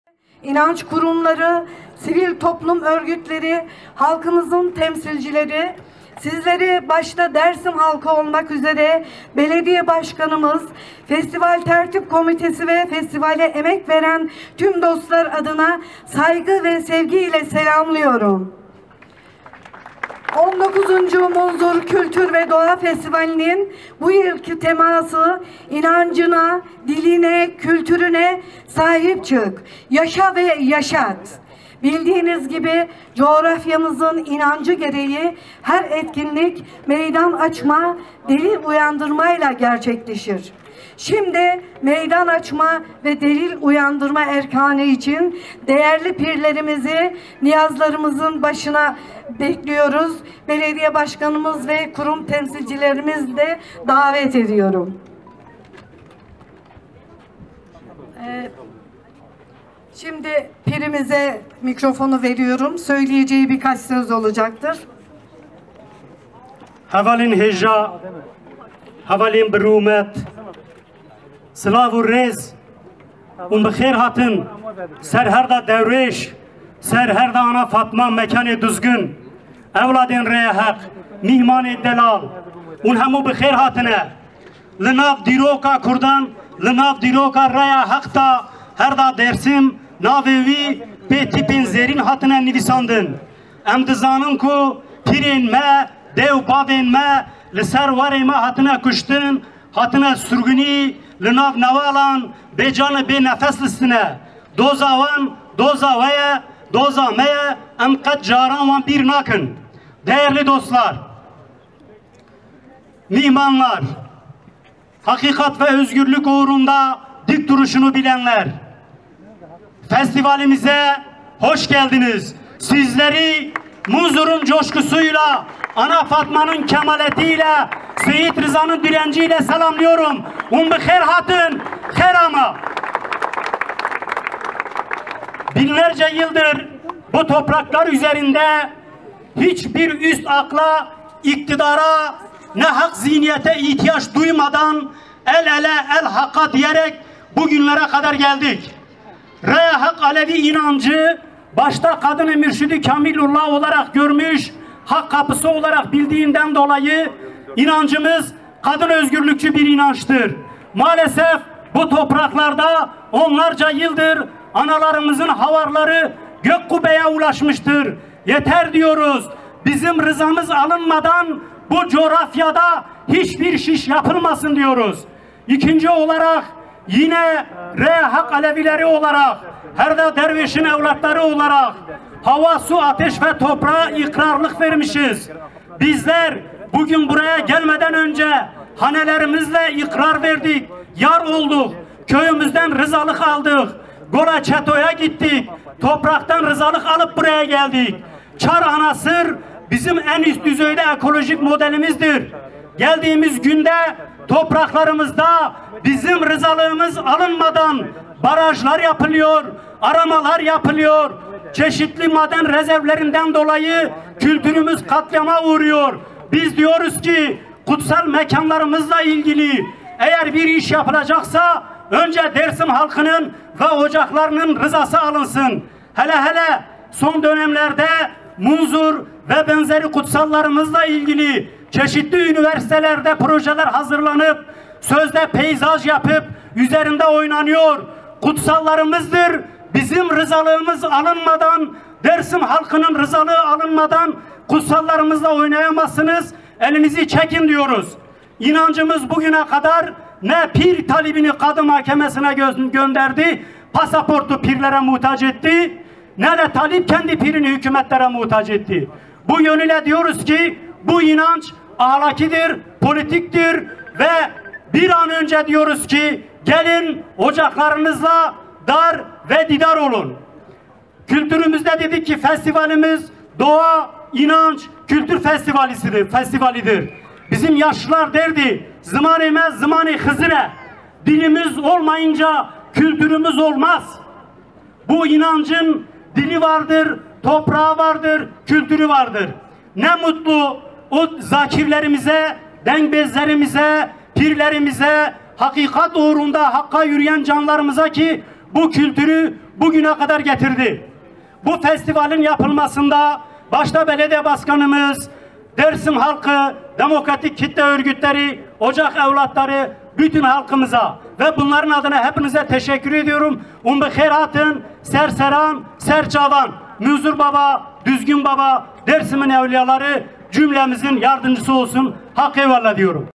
Festival Başladı
19. Munzur Kültür ve Doğa Festivalinin meydan açma töreni ve çıla açma gerçekleşti.